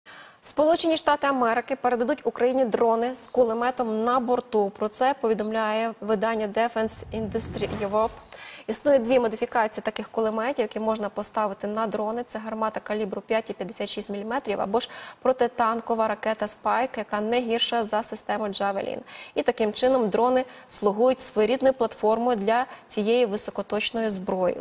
США передадуть Україні ударні дрони Felon з кулеметами та ракетами на борту
США передадуть Україні передові ударні дрони з вогнепальною зброєю на борту виробництва Feloni Aero.